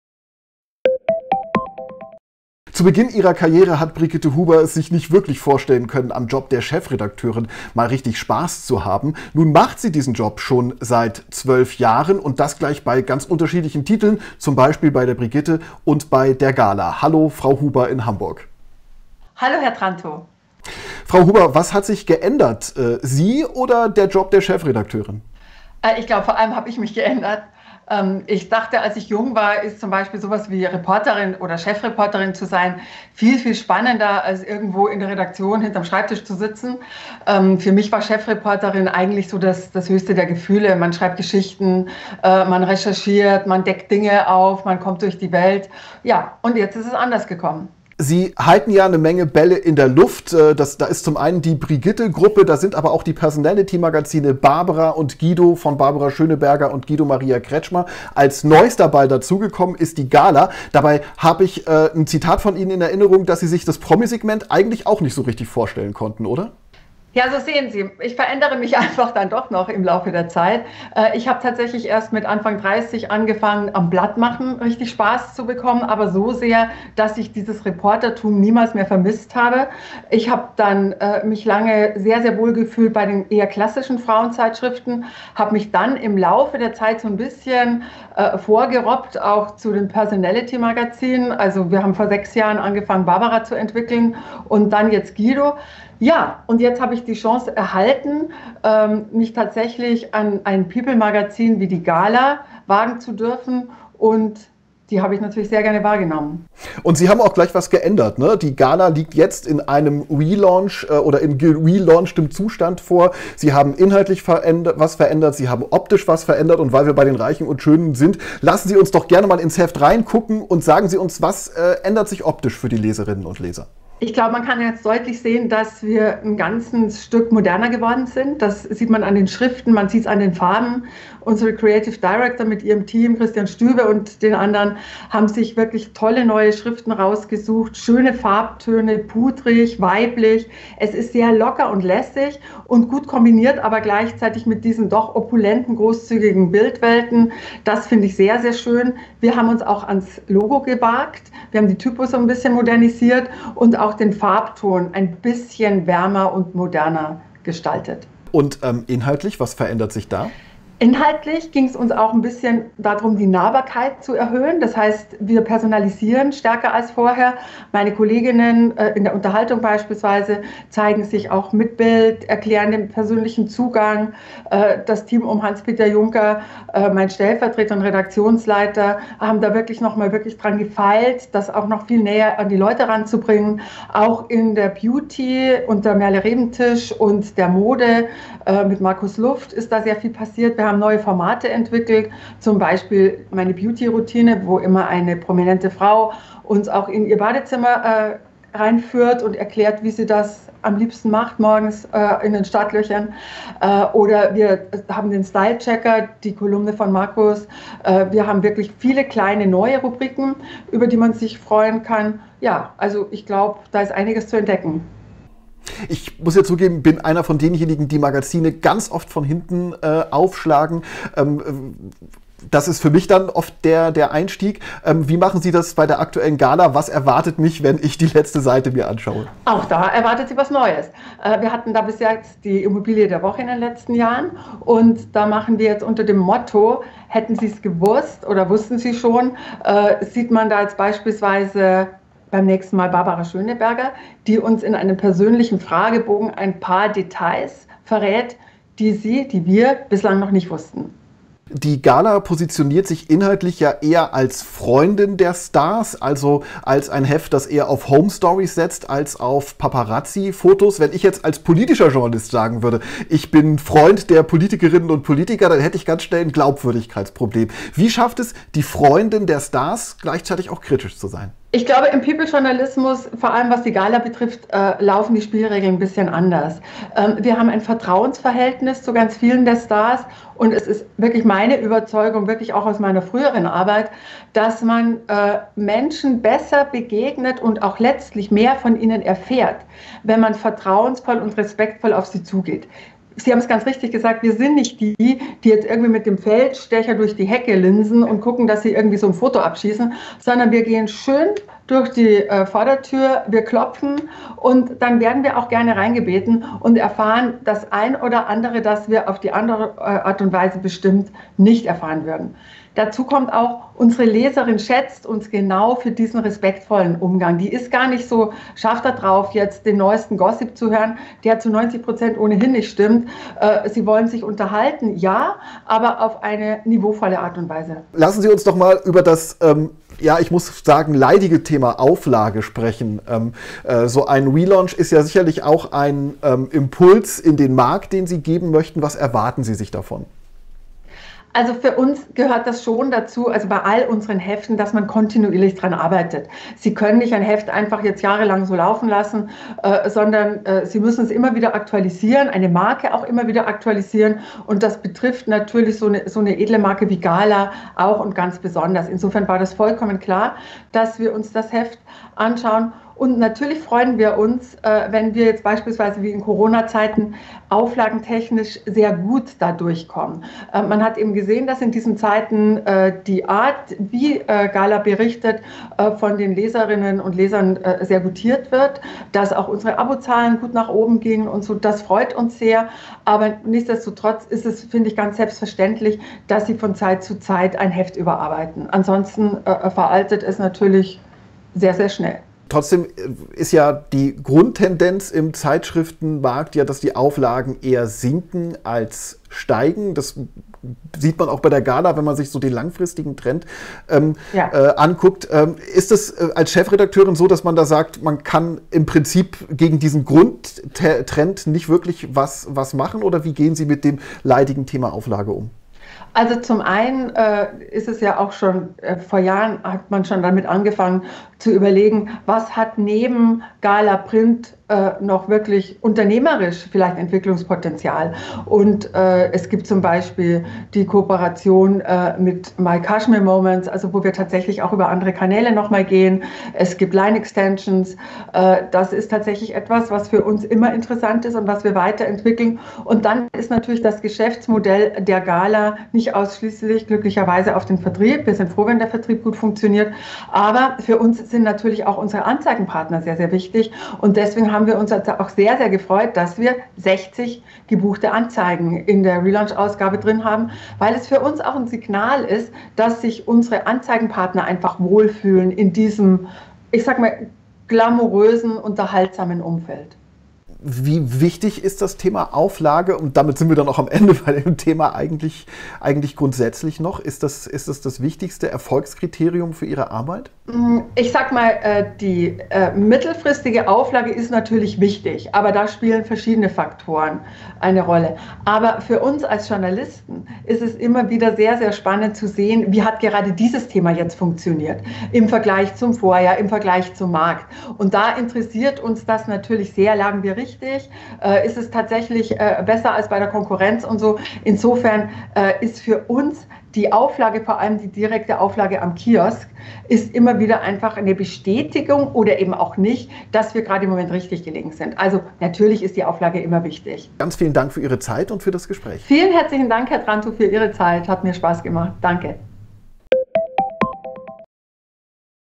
“Wir gehen durch die Vordertür” – Chefredakteurin Brigitte Huber zeigt die neue “Gala”.
Alles neu macht der März: Das People-Magazin "Gala" liegt seit heute in runderneuerter Form am Kiosk und im Briefkasten der Abonnentinnen – im Video- und Podcast-Interview von turi2 zeigt und erklärt Chefredakteurin Brigitte Huber, was sie und ihr Team verändert haben. So ist das Logo jetzt moderner, die Farben sind frischer und die Bildstrecken großzügiger. Auch inhaltlich baut Huber ordentlich um: Die Schluss-Rubrik "Immobilie der Woche" muss weichen – künftig verrät am Ende immer eine Prominente ein bisher unbekanntes Detail aus ihrem Leben. Auch im Heft gibt es neue Rubriken, die die Leserinnen näher an die Promis heranbringen sollen – ohne aufdringlich zu sein.

Die "Gala" gehe durch die Vordertür und liegt nicht mit dem Feldstecher auf der Lauer, sagt Huber. Sie ist überzeugt, dass sie in einem Vertrauensverhältnis zu den Prominenten mehr erfährt als durch Gerüchte, die am Ende ohnehin zu 80 % falsch seien. Die Leserinnen wüssten das zu schätzen. Huber erklärt, dass sich die Abo-Zahlen in der Pandemie gut entwickelt haben und dass das Heft auch bei den Werbekunden gut ankommt – die Chefredakteurin freut sich über "60 gebuchte Anzeigen" in der Relaunch-Ausgabe.

Im ausführlicheren Podcast spricht Huber darüber, wie sie in die Rolle der Multi-Chefredakteurin hineingewachsen ist. Huber betreut neben der "Gala" die "Brigitte"-Gruppe mit mehreren Magazinen sowie die Personality-Magazine "Guido" und "Barbara". Sie hat das Loslassen lernen müssen, sagt Huber, leicht gefallen ist ihr das anfangs nicht.